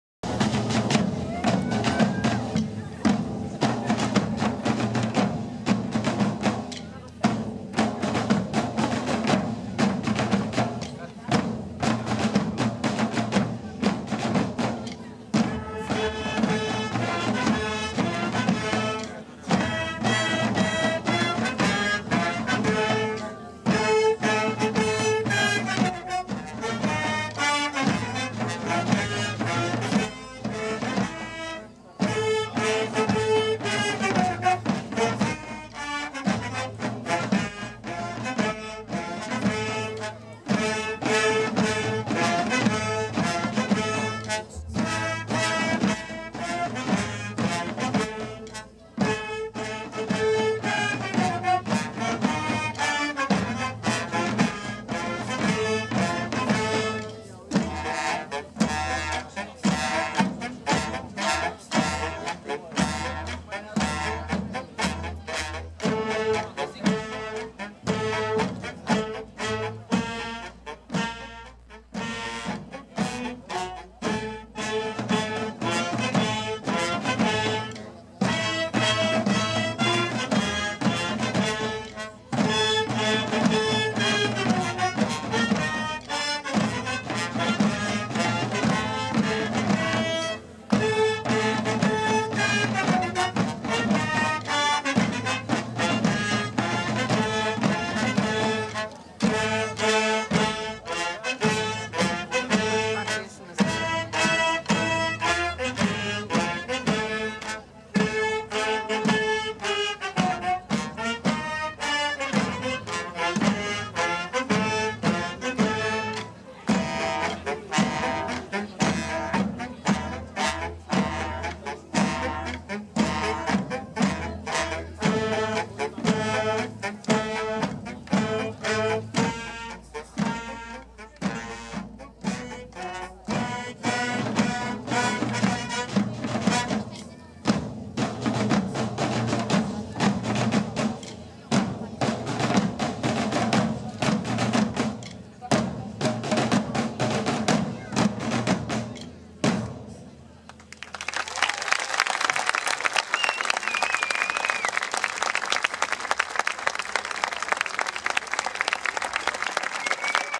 Festas da Cidade 2014, Orquestra Ligeira de Ponte de Sor e Batucando Orquestra de Percussão
Festas da Cidade 2014, Ponte de Sor